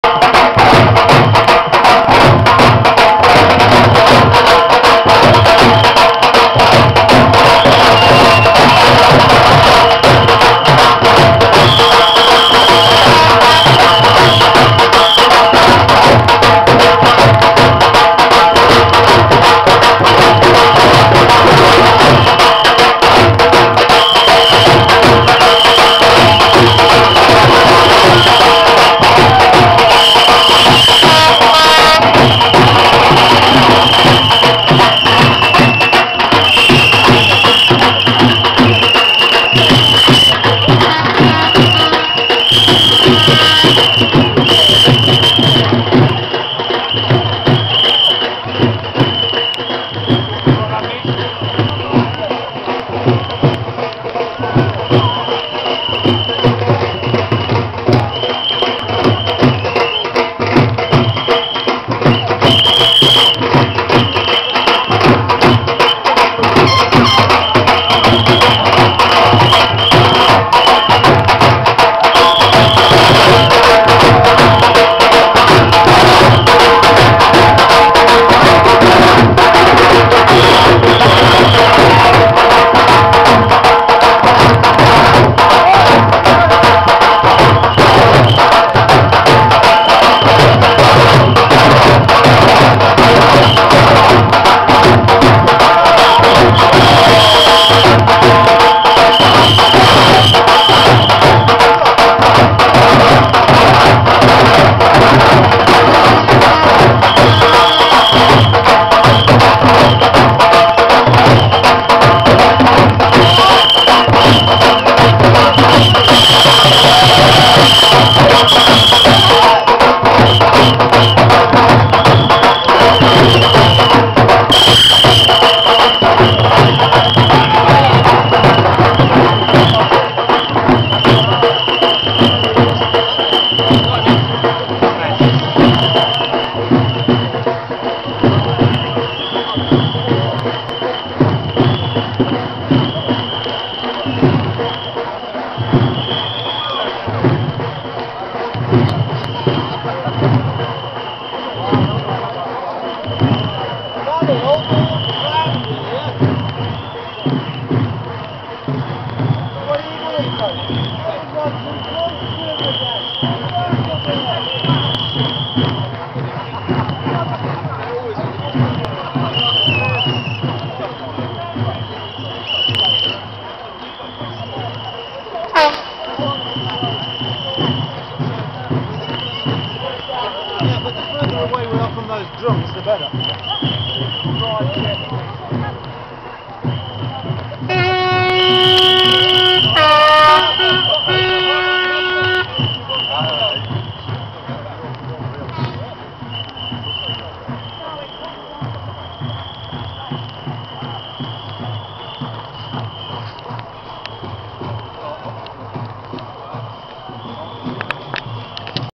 Save Milton Keynes Mail Centre Demo
Communication Workers' Union organised demonstration against the closure of MK Mail Centre and to secure jobs,terms and conditions for all postal workers facing closures.Demo was held in Bletchley,Milton Keynes on 15th.Postal workers from all over attended as did other workers' unions. November 2008.Bhangra drumming group was the excellent Dholl By Dholl.